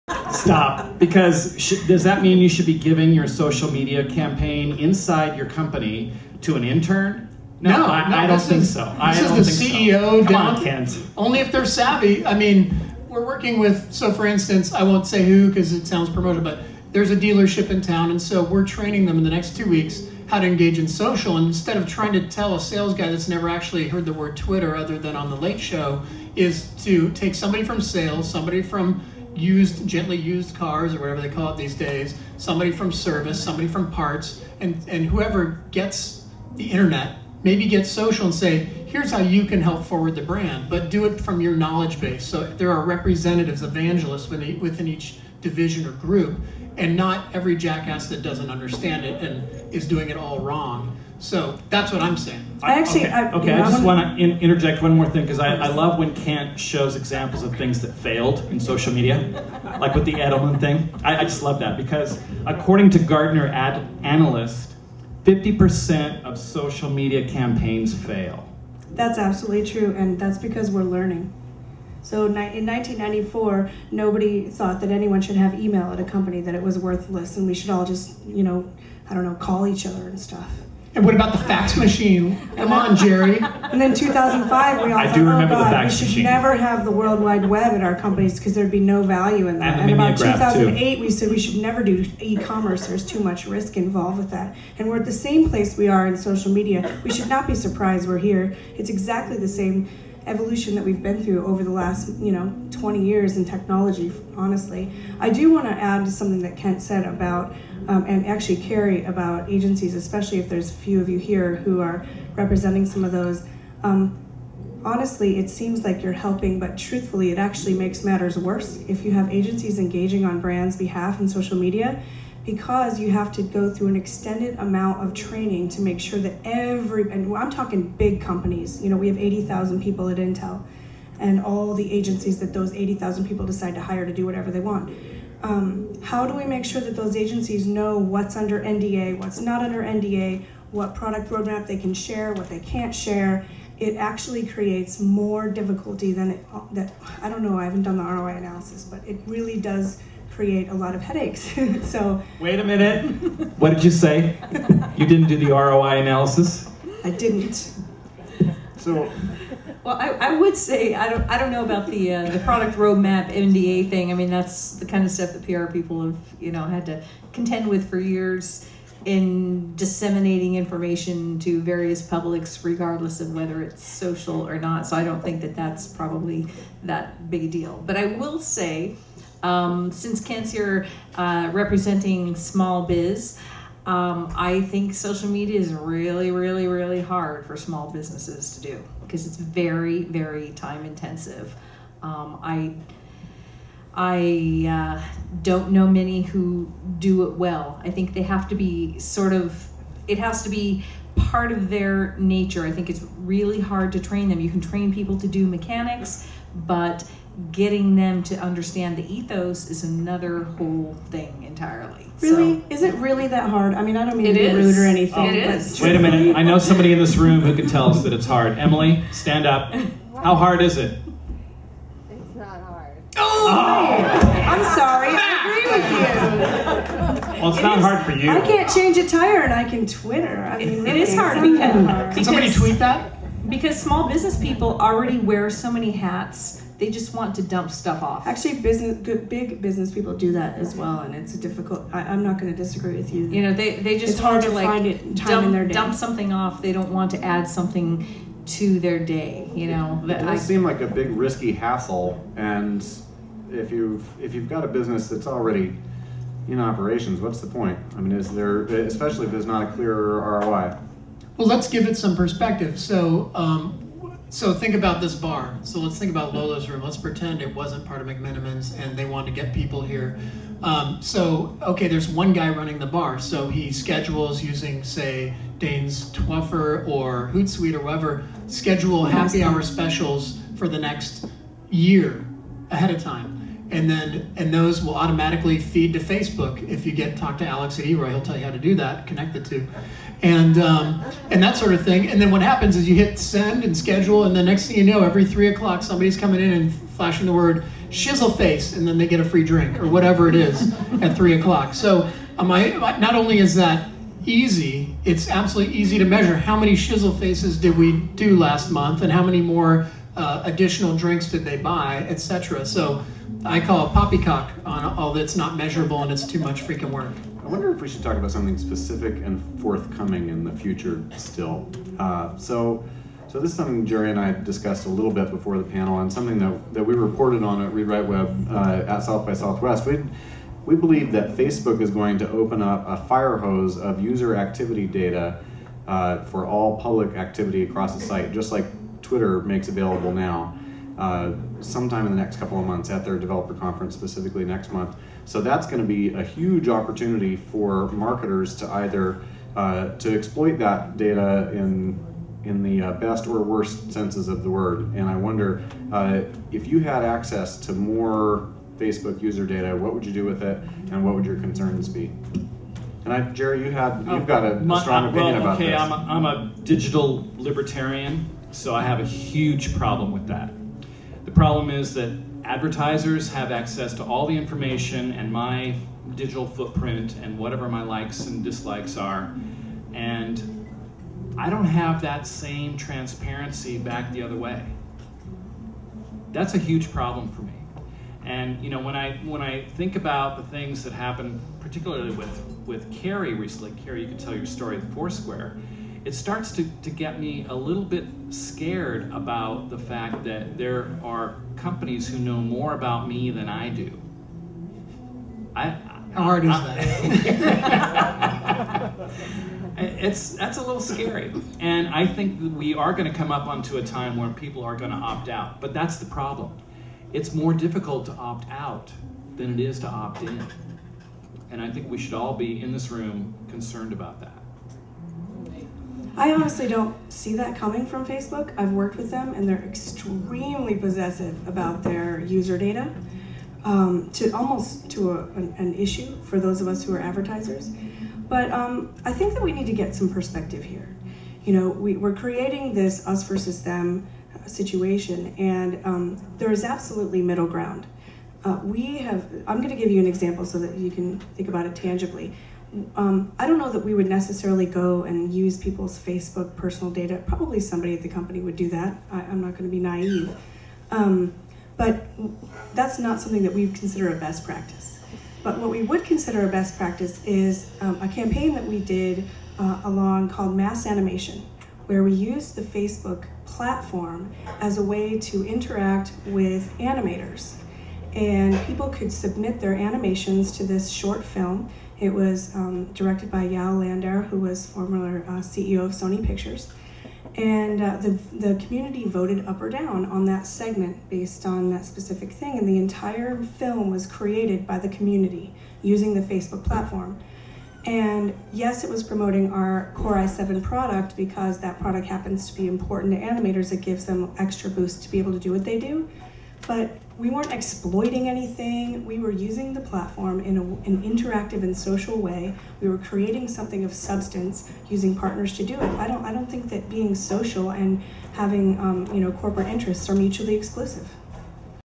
Portland Advertising Federation 2010 - Debate - Social Media - #3
Join Marshall Kirpatrick of Read Write Web website as he moderates a lively discussion on the pros and cons of Social Media today. In the Pro corner will be Kelly Feller, Social Media Strategist at Intel and Kent Lewis of Anvil Media. In the Con corner will be Jerry Ketel of Leopold Ketel & Partners and Carri Bugbee, an independent PR and marketing professional. Should brands invade the social space? Do the metrics of social media translate to ROI? Find out!  This video is 3 of 7.